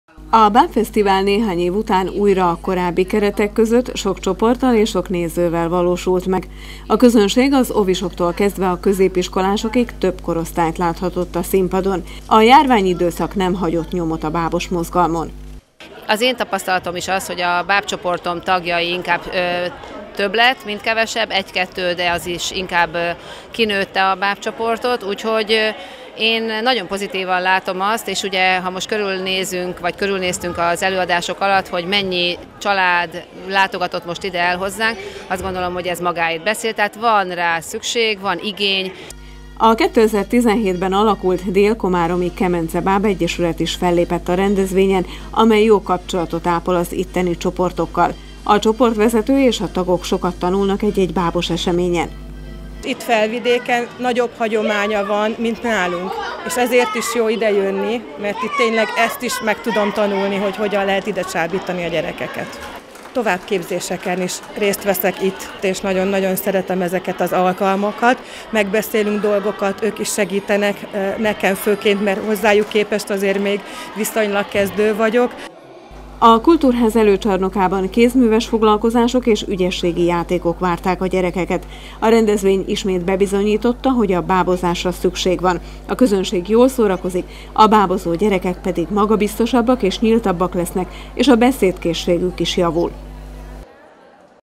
Sok csoport és sok néző a bábfesztiválon
A városi művelődési központban tizenhatodszor rendezték meg a Tarka Lepke Bábfesztivált. A közönség a Gézengúzok, Mazsola, Tekergők, Futrinka, Tátika és Iciri-Piciri bábcsoportok, valamint a Mesevarázs Bábszínház, a Kemence Bábegyesület és a galántai Kodály Zoltán Gimnázium bábcsoportjának produkcióját láthatta.